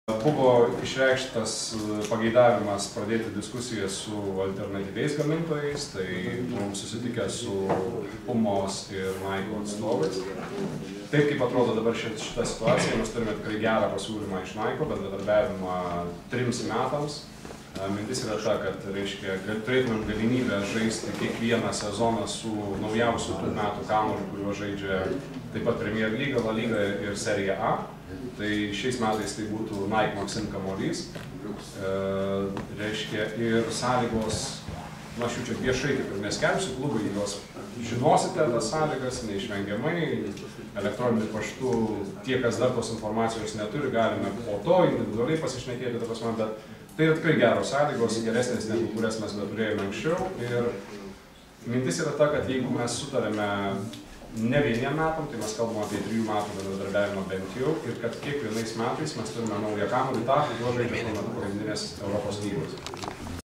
LFF A lygoje 2013 m. numatoma žaisti NIKE kamuoliais
Kol kas galutinai nepatvirtinta, tačiau oficiali informacija, išsakyta LFF konferencijoje